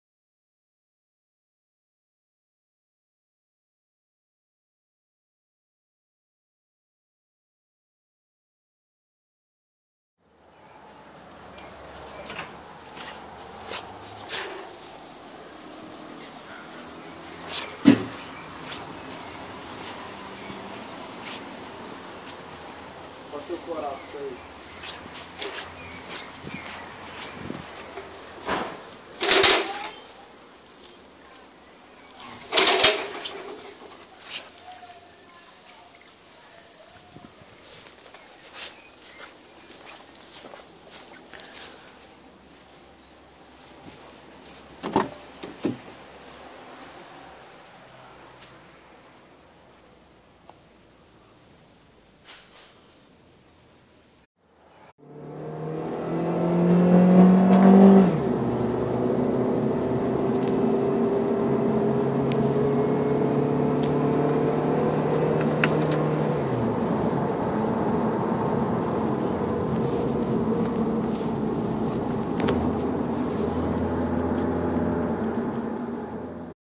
Civic 2013 1.8
Subindo a serra, motor fraco